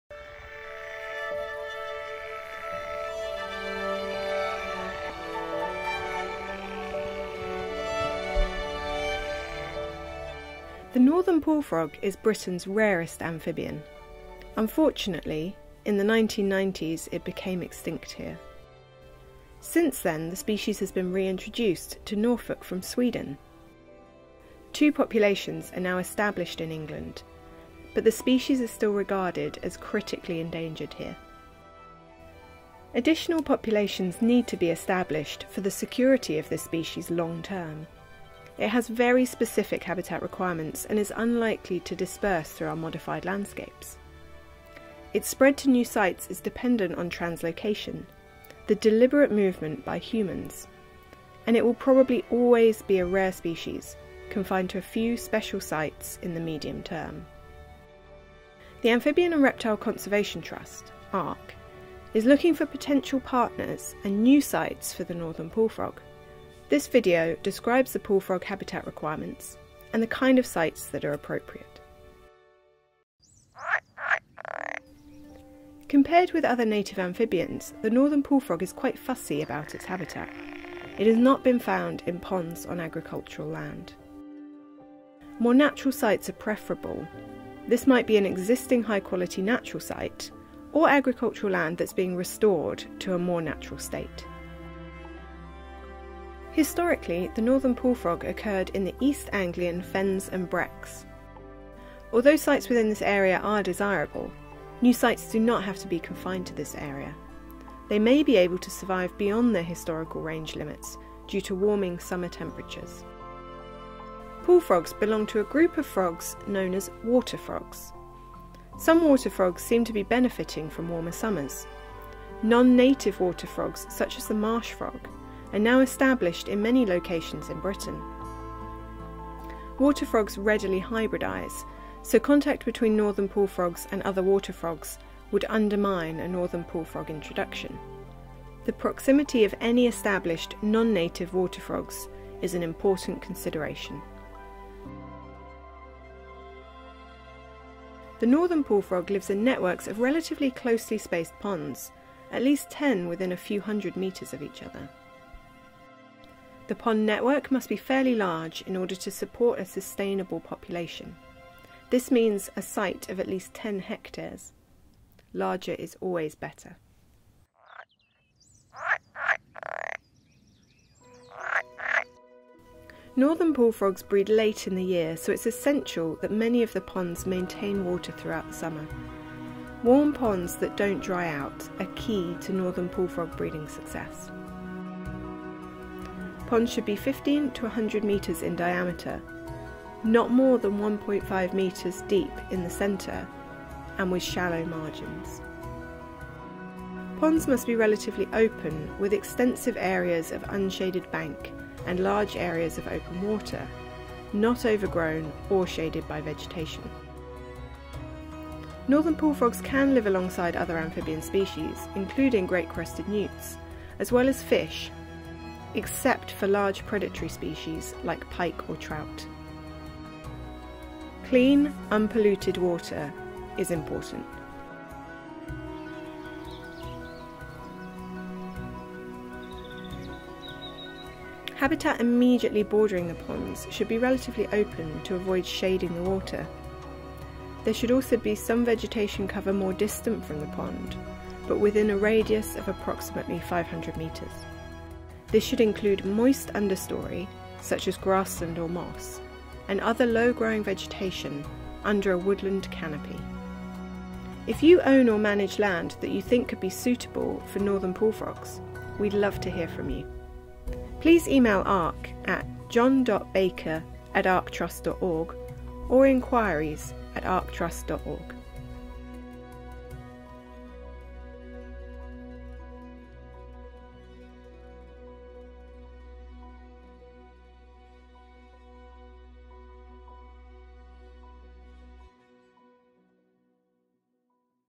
Northern Pool Frog Site Specification
Amphibian and Reptile Conservation is looking for new homes for the northern pool frog, but this rare species is demanding in its habitat requirements. To help decide whether you could help with pool frog recovery, we have produced information on what sort of habitat is needed.

Music composed by Rob Hicks and Tristan Noon

This project is funded by the Government's Green Recovery Challenge Fund. The fund was developed by Defra and its Arm's-Length Bodies.